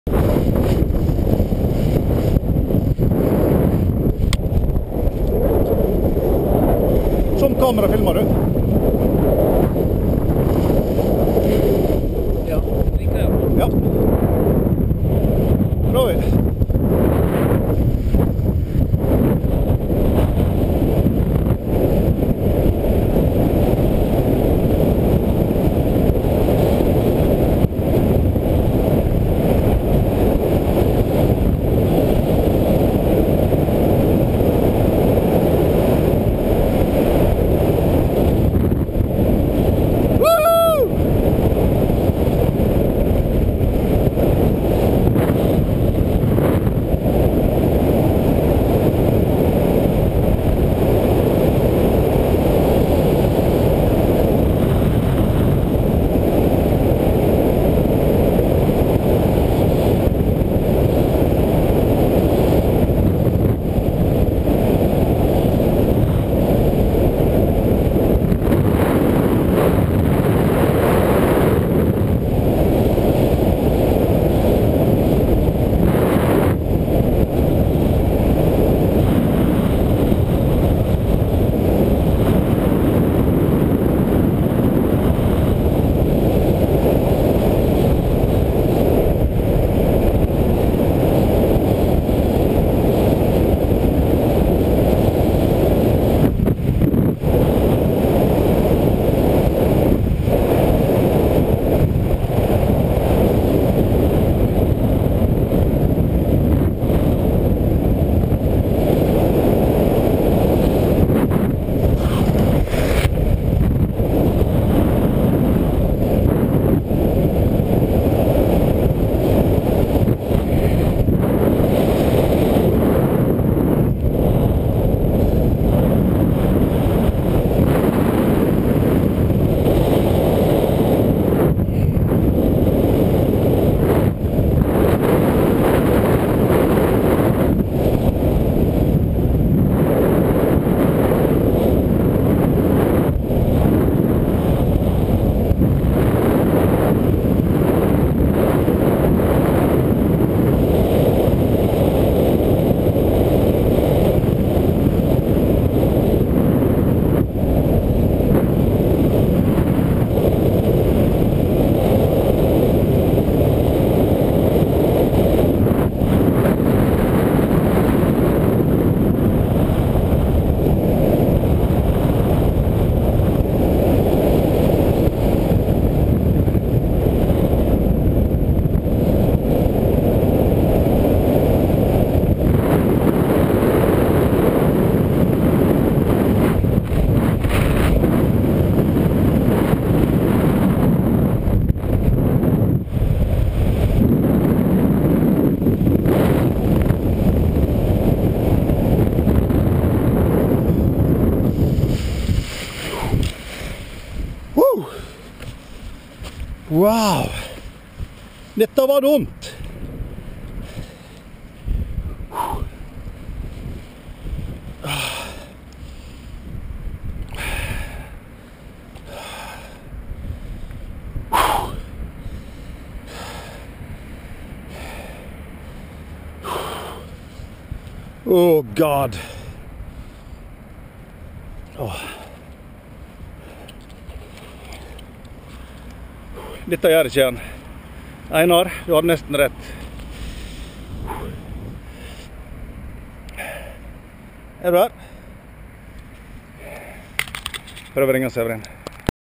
Mickey Speedgliding Waves Skafjellet - Bedehuset Totalcollapse
Severin Eilertsen and me went up to Skafjellet to put up a new windsock. Strong wind straight in. (7-8 m/s) Lenticularis on the sky. Expected worst case a treelanding since it is quite flat.

Flying my Swing Mirage 13,5 @+90kg all up.

Took off, climbed, then sink. Experimented with open trimmers. Closed them and had control. Severin saw that I landed since the glider dropped down behind me. Total collapse, quick on the brakes and reopened. Thought I'd land by the antenna, then by the school. Got lift and made it almost back to the main landing??
Very stupid. Lessons learned. A speedglider cannot take everything..
Thanks to Severin for beeing standby in case of a treelanding :)